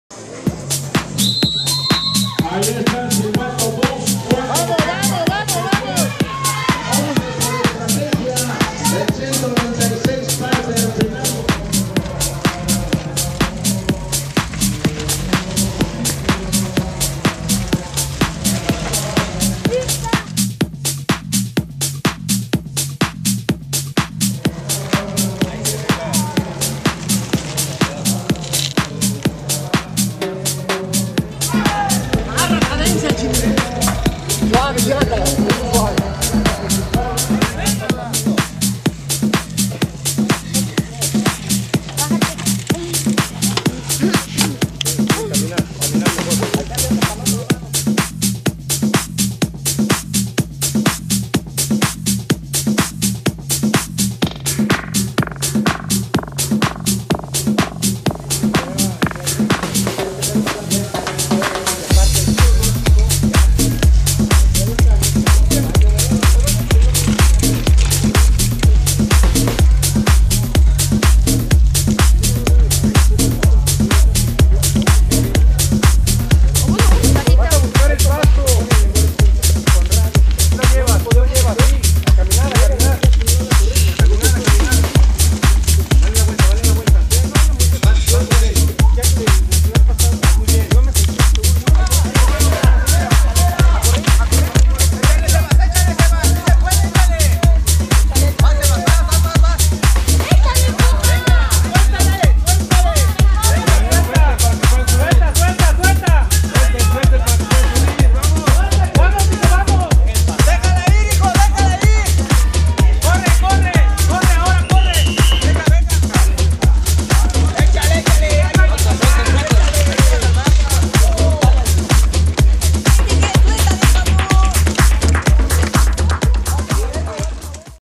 XCO Mountain Bike Valle de Bravo 2017 Serial Nacional Infantil
Serial nacional de mountain bike en México, Valle de Bravo
Compilación infantil.
Vinder Guide